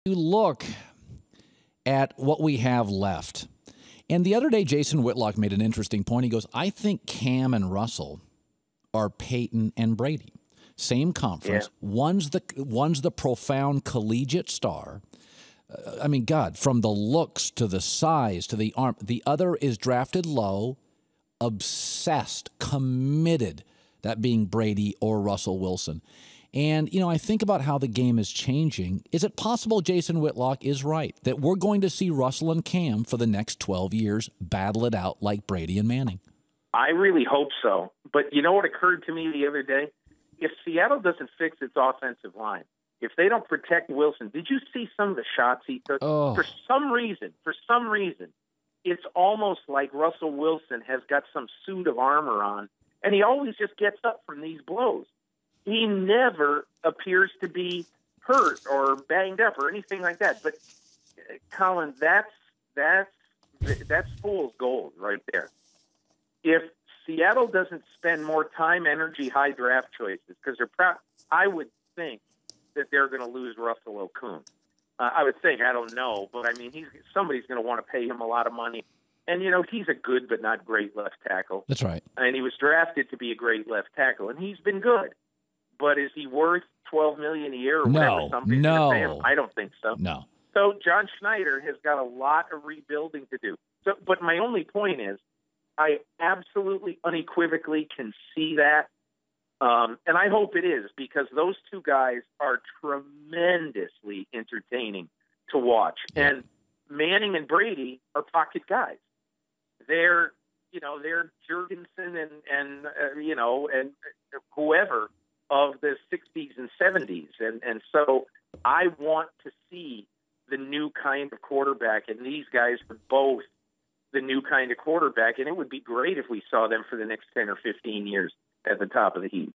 Here's the one thing that could keep Wilson/Newton from being Brady/Manning | THE HERD
Peter King knows one thing that could keep Wilson/Newton from being the next Brady/Manning.